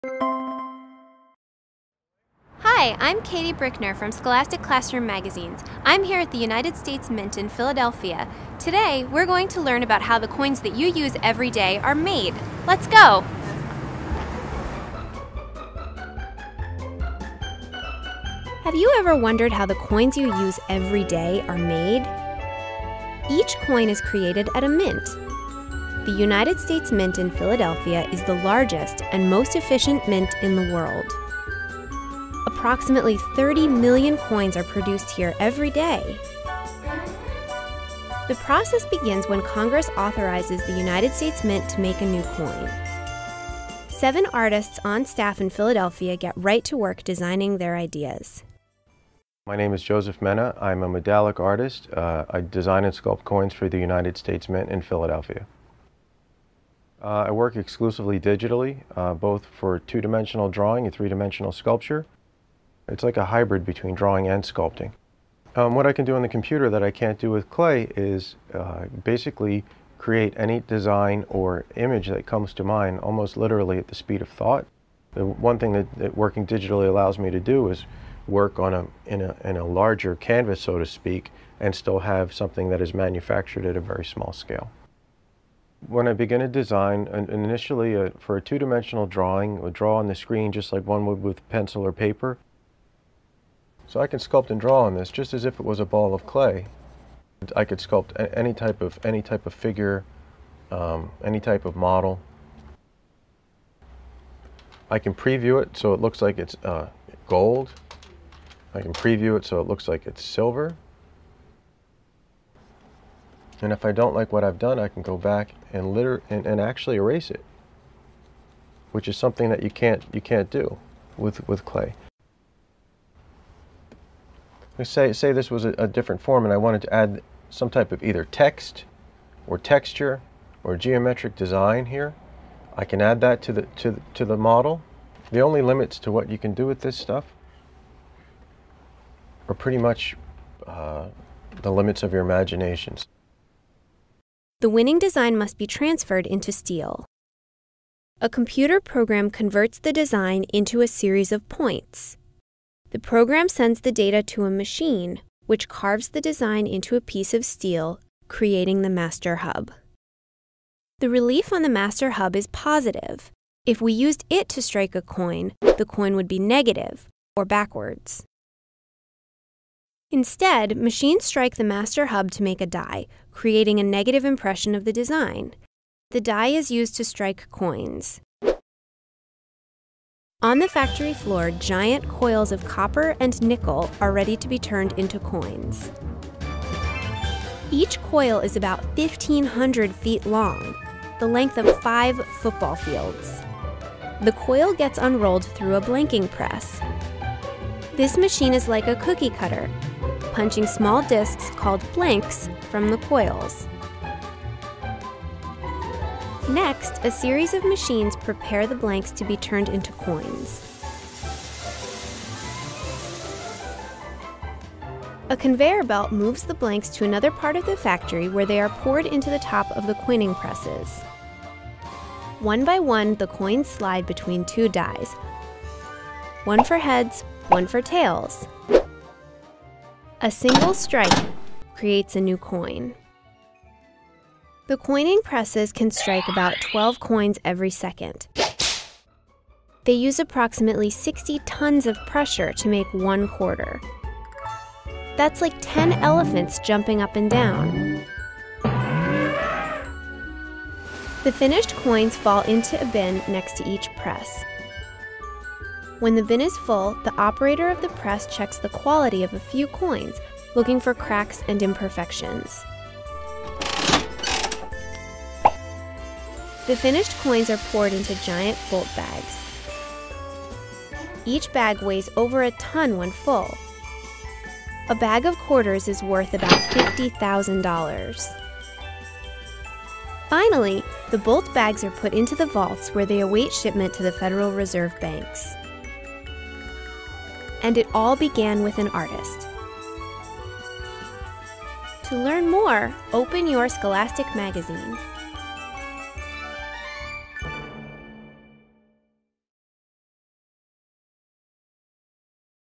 How Coins Are Made
Scholastic Video about how coins are made in the USA.